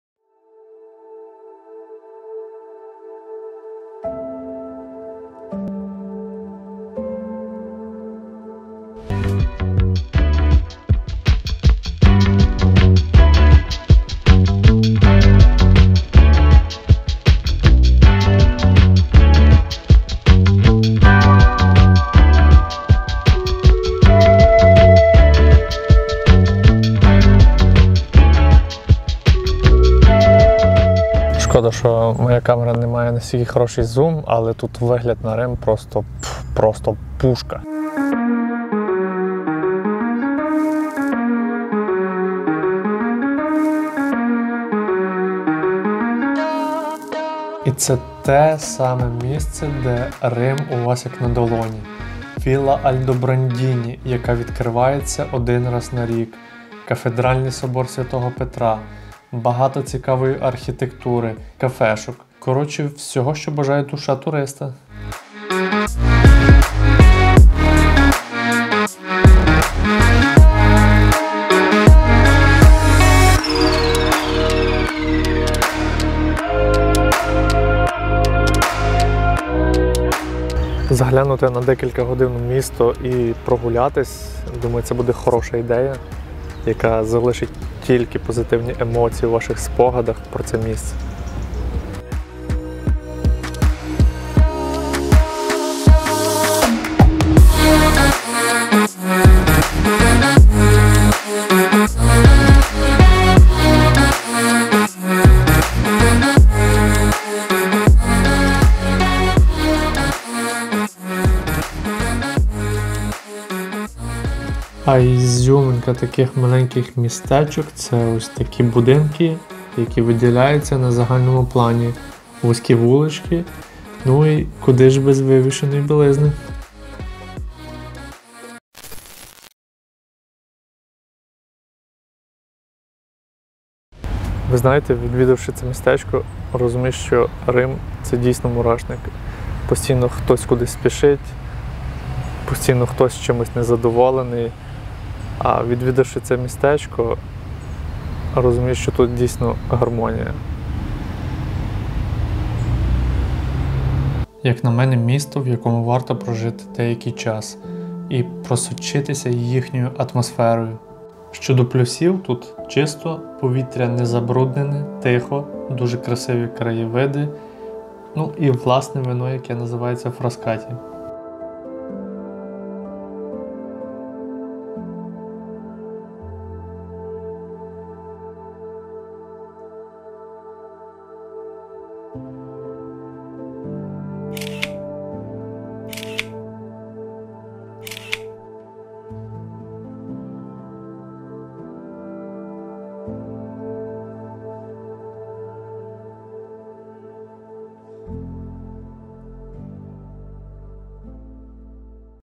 Frascati #12. View of Rome. Traveling during the Pandemic. [ JustPPD ]
З того що я бачив за весь час, це місце є найкращим щоб роздивитись Рим. Тут відкривається непогана панорама на Вічне місто, окрім цього прогулянка по невеличкому містечку з його особливою архітектурою та людьми. Вас очікує тут вілла Альдобрандіні, кафедральний собор св. Петра, багато вуличок для дослідження та обов'язково скуштуйте вино "Фраскаті".
From what I've seen all along, this place is the best to see Rome. It offers a good view of the Eternal City, in addition to a walk through a small town with its special architecture and people. Villa Aldobrandini, the Cathedral of St. Peter, many streets to explore and be sure to taste the wine "Frascati".